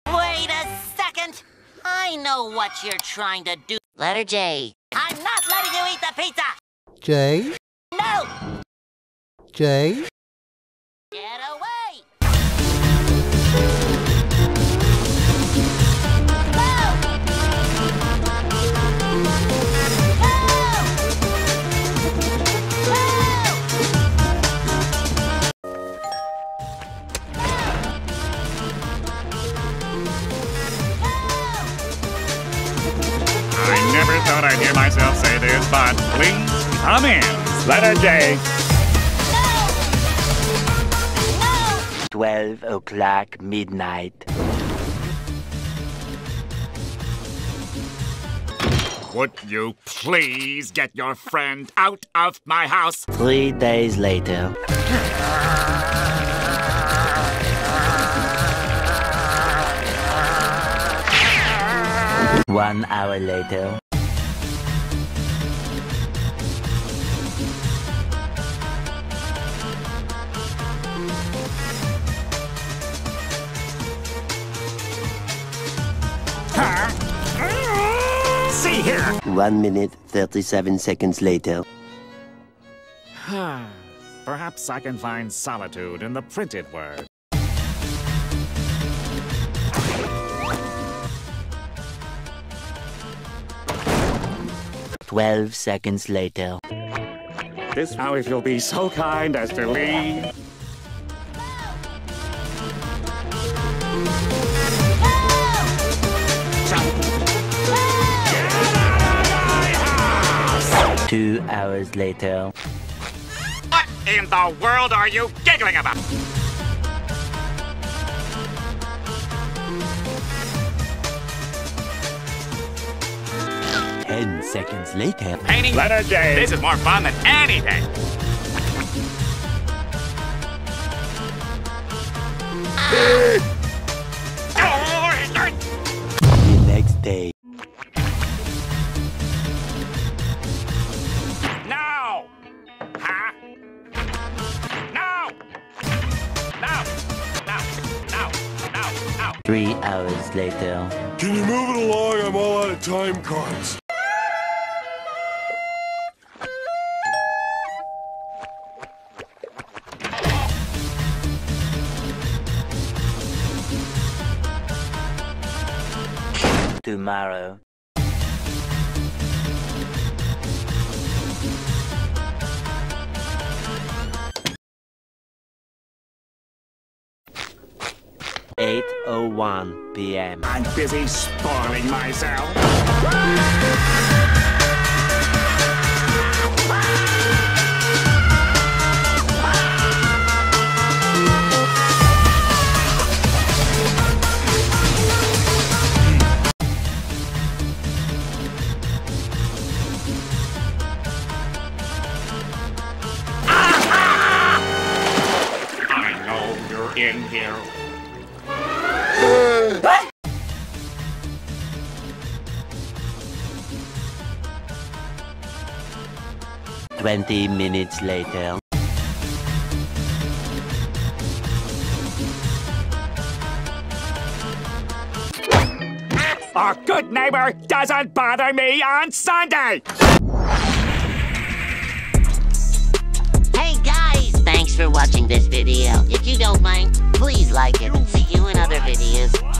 Squidward kicks Dr Livesey Walk Alphabet Lore Letter J out of his house
Letter J of the English Alphabet is feeling lonely and left out. She's been trying to make friends but none of her other letters seem to understand her. So she decides to take a walk and explore the neighborhood.

After a long walk, she finds herself in front of Squidward's house. She decides to knock on the door and introduce herself.

Unfortunately, Squidward is a bit of a grump and doesn't want to let Letter J in. He tells her to go away and slams the door shut.